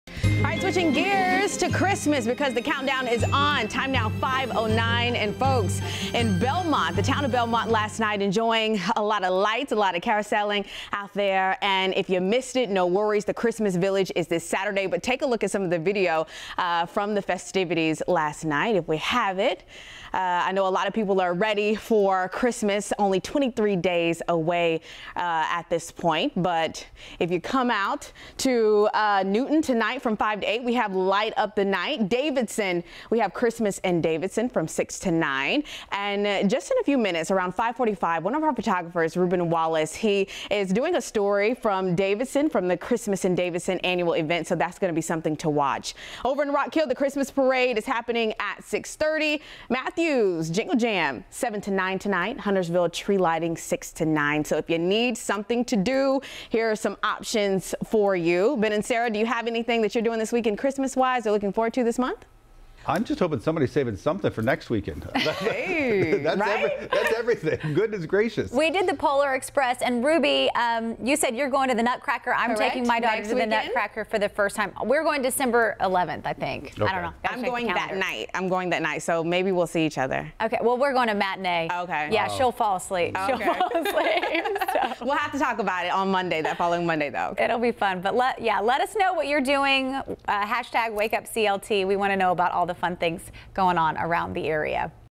CLT Happenings: 21 things to do this weekend in Charlotte
It’s officially the Christmas season, and that means there are plenty of things to do to get you in the holiday spirit this weekend in the Queen City.

See what’s happening!

Food Truck Friday
Come out for good eats, live music and fun for the whole family at various Food Truck Friday events happening all over the Queen City. Come out to get your grub on at Food Truck Friday at Cabarrus Brewing Company, University City Food Truck Friday, or Food Truck Friday at Sycamore Brewing. Times vary by location.